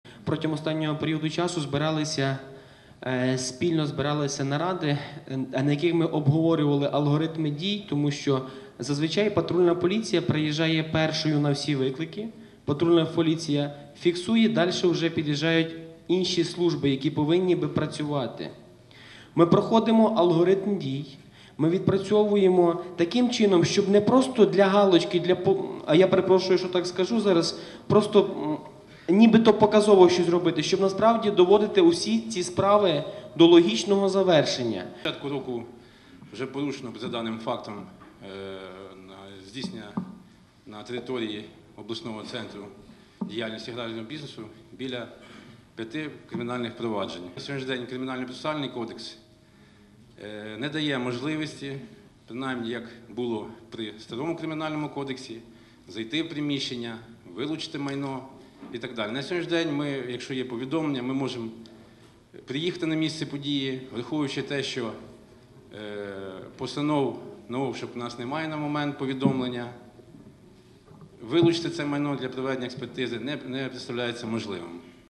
Поліцейські прокоментували, як борються із гральним бінесом у Чернівцях
Олег Йосипів, начальник управління патрульної поліції Чернівців6 Протягом останнього періоду збирались спільні наради, під час якого ми проговорювали спільні алгоритми дій, щоб доводити усі ці справи до логічного завершення.
Василь Паращук, начальник Чернівецького відділу поліції: З початку року вже порушено за цим фактом здійснення на території Чернівців грального бізнесу. Разом з цим ця функція відноситься до підрозділу управління захисту економіки. Кримінально-процесуальний кодекс не дає можливості зайти в приміщення, вилучити майно.